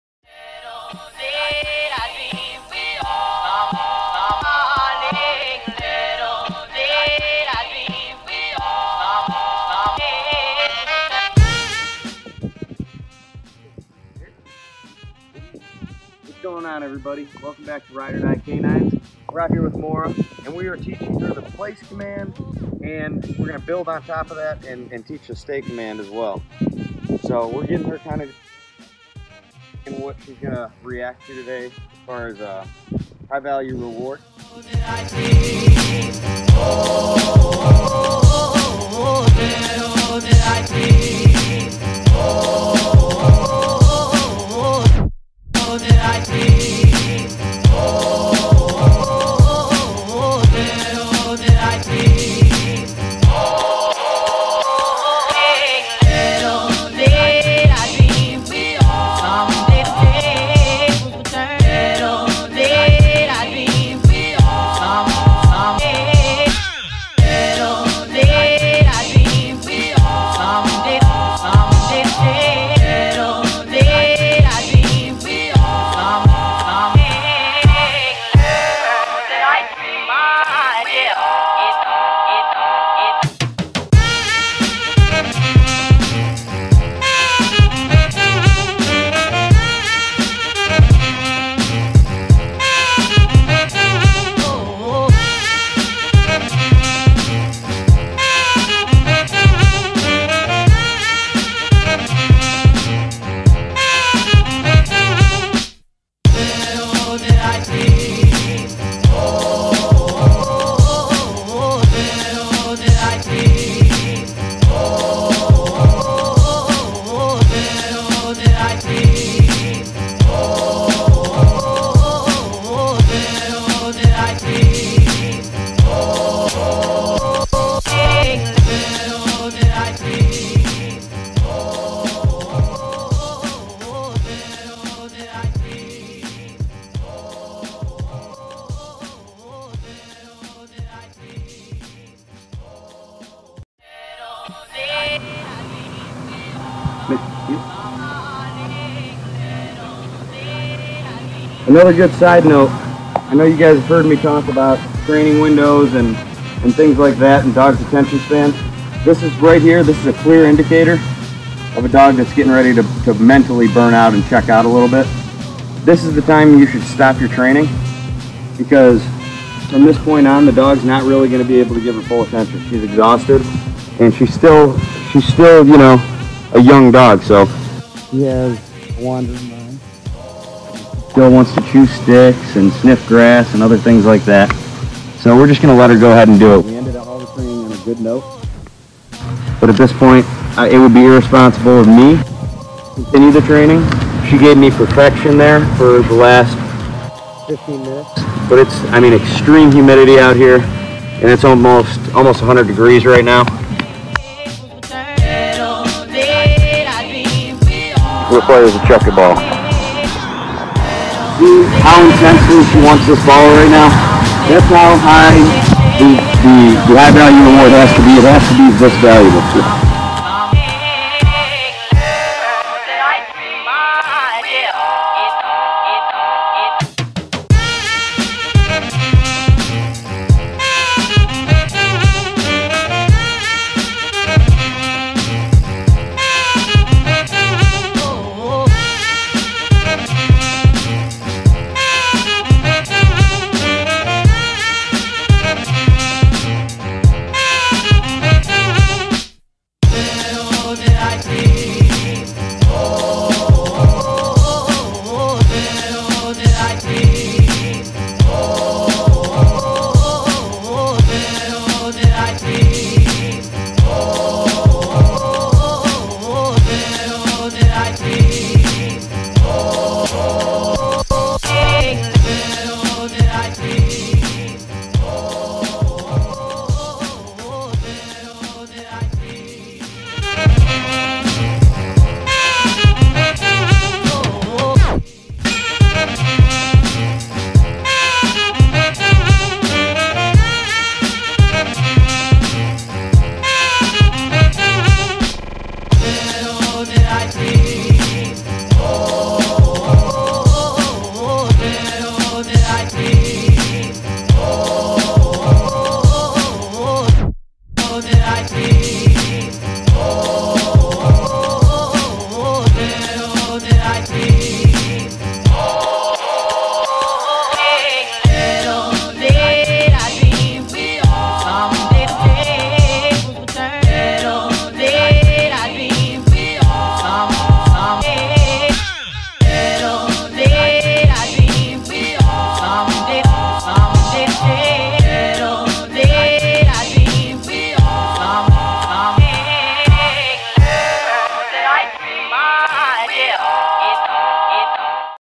Knowing When - Training ‘Time Out'
Understanding your dogs drive, and what type of reward gives them the most intensity during your training sessions is just as crucial as understanding when your dog is tired and won’t work anymore — Training Window.